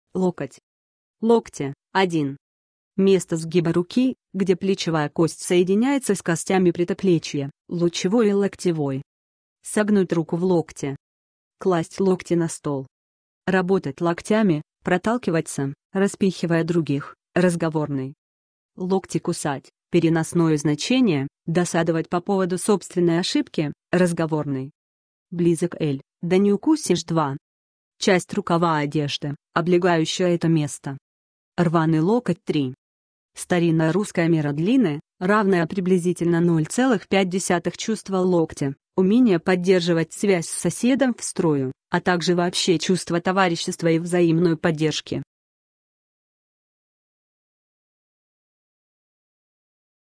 ЛОКОТЬ - что это такое? значение и описание
Хочешь знать что значит ЛОКОТЬ? тогда слушай до конца. n n -ктя, мн. локти, -ей, м. 1. Место сгиба руки, где плечевая кость соединяется с костями предплечья (лучевой и локтевой). Согнуть руку в локте. Класть локти на стол. Работать локтями (проталкиваться, распихивая других; разг.). Локти кусать (перен.: досадовать по поводу собственной ошибки; разг.). Близок л., да не укусишь (посл.). 2. Часть рукава одежды, облегающая это место. Рваный л. 3. Старинная русская мера длины, равная приблизительно 0,5 м. * Чувство локтя - умение поддерживать связь с соседом в строю, а также вообще чувство товарищества и взаимной поддержки. || уменьш. локоток, -тка, м. (к 1 и 2 знач.). || прил. локтевой, -ая, -ое (к 1 знач.). n n Больше слов